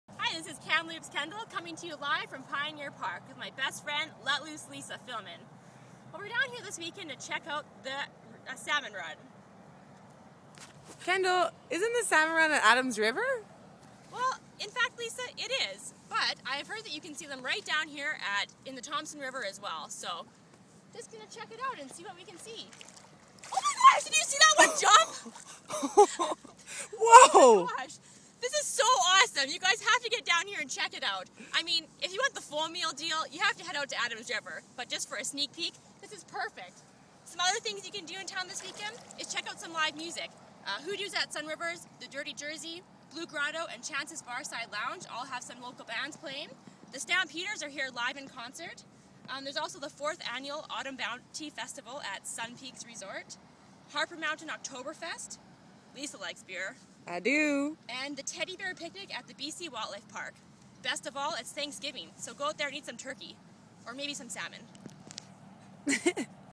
Around Town with Kamloops Kendel and Let Loose Lisa!
Thanksgiving weekend in Kamloops is full of stuff to do!  Watch Kamloops Kendel and Let Loose Lisa as they discover the Salmon Run in Kamloops,.  Check back for weekly updates and remember... let loose and just play in Kamloops, BC!